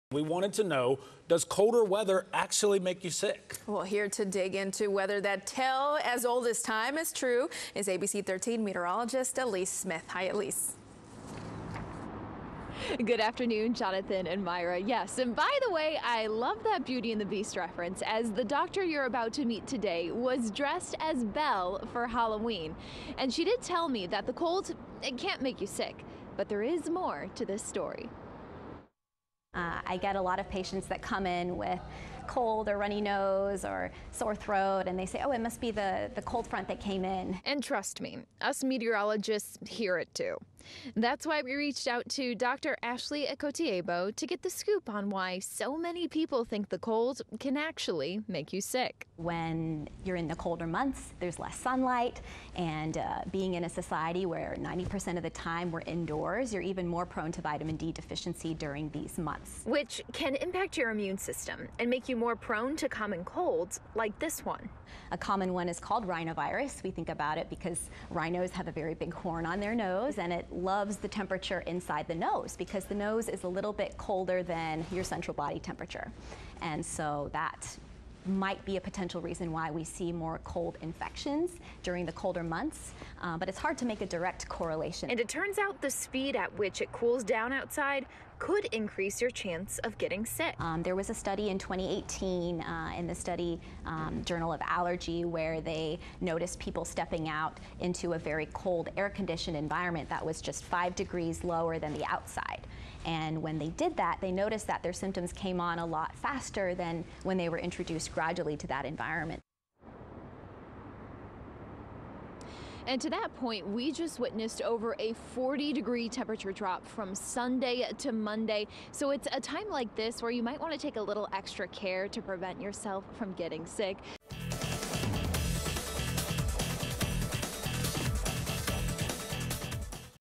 Mythbuster: Can cold weather make you sick? Here's what we found
Is there a correlation between cold weather and being sick? A Houston doctor says we might see more people sick during the colder months, but it's hard to make a connection.